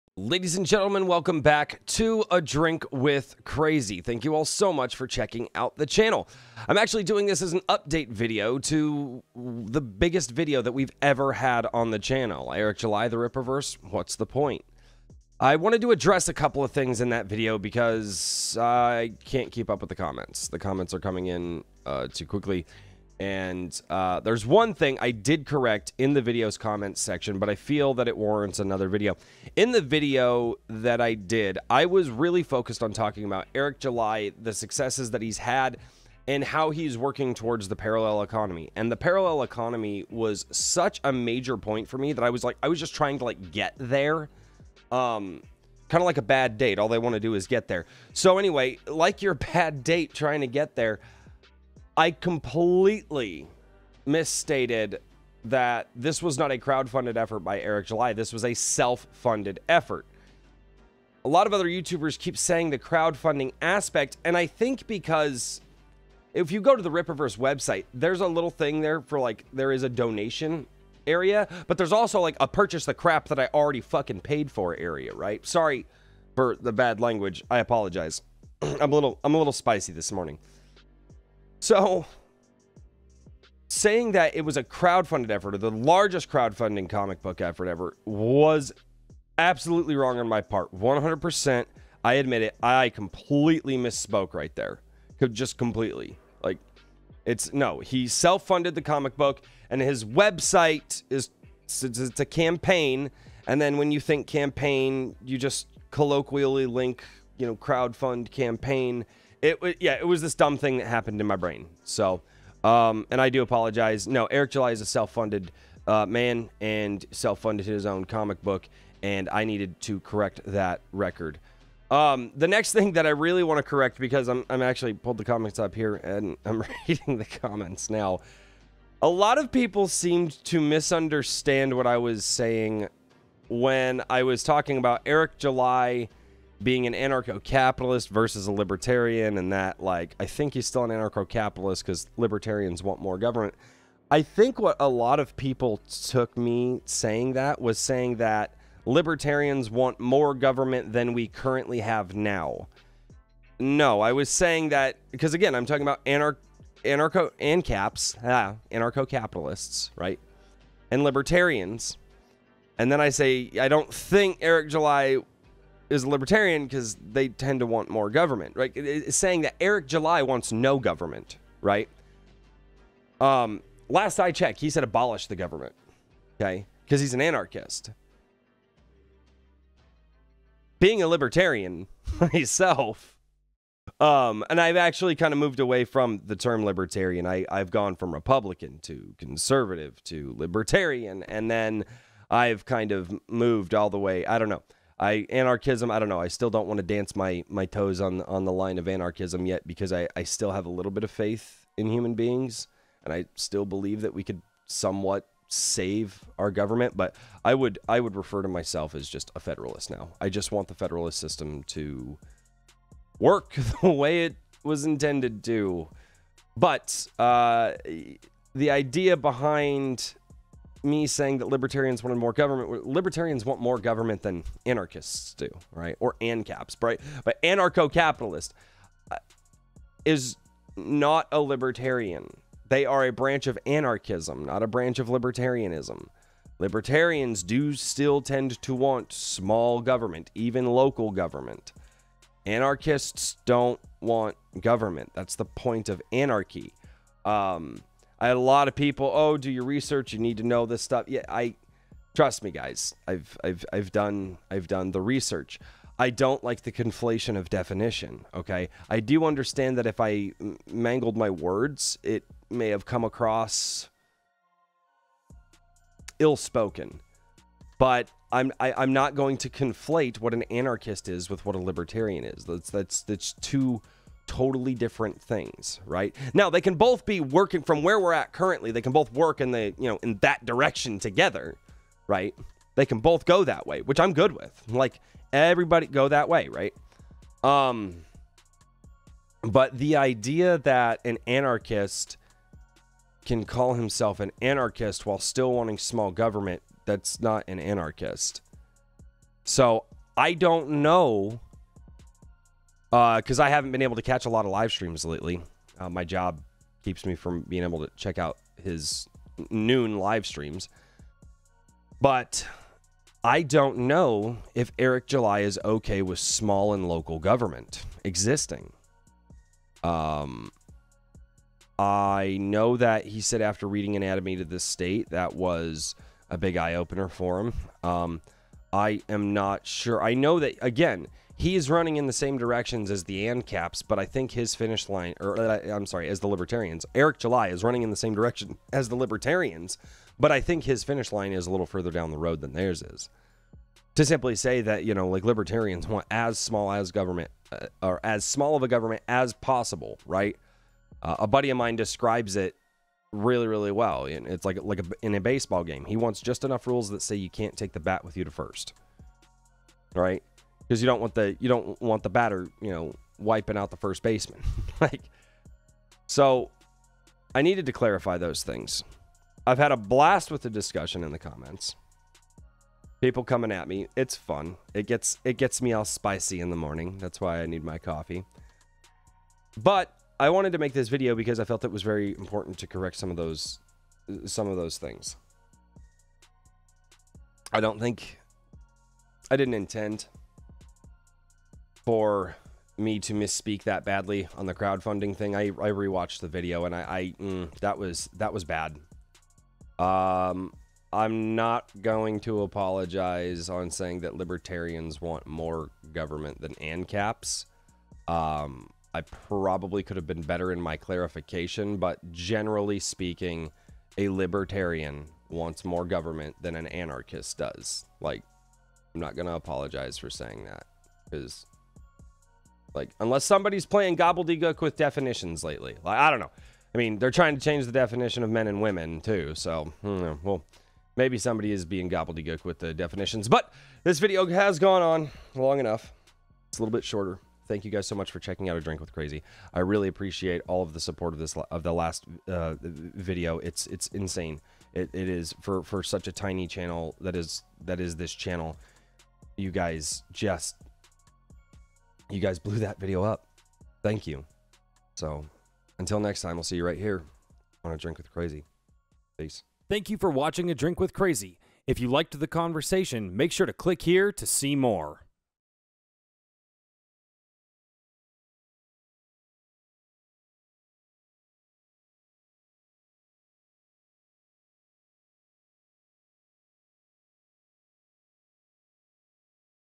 Eric July - The Rippavers -What's the point? Pt. 2 Correcting my mistakes
Today's Topics: After the amazing response to Eric July The Rippavers- What's the point? I felt that the discussion could go further than the original video. I made some mistakes and wanted to offer some clarification.
Check us out on Twitch Every Saturday:
Twitch.tv/adrinkwithcrazy